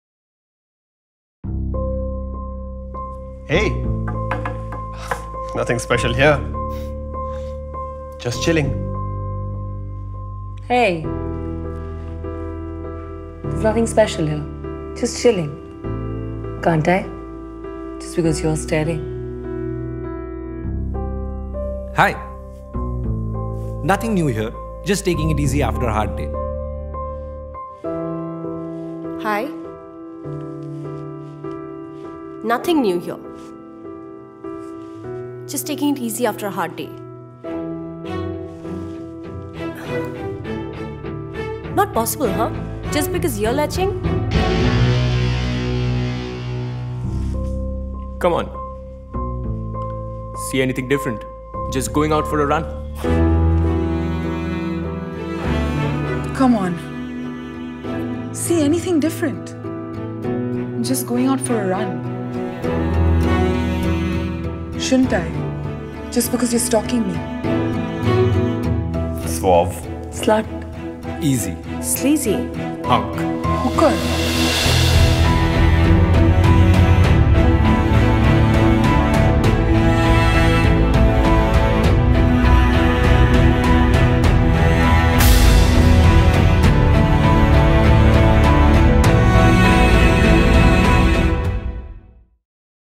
Turtle Relook
In India, where gender bias is a big social anomaly, women face grave inequalities everywhere, almost every day. And what they wear is just one aspect of this. While men have the right to wear anything under the sun, there isn’t an outfit that a woman wears without being judged by men.
On International Women's Day, Turtle Menswear wanted to address this issue and create a conversation around it from the perspective of fashion through a hard-hitting video – the freedom to dress the way one wants, without being judged by one’s gender.

Created by Turtle Limited
Conceptualized by J Walter Thompson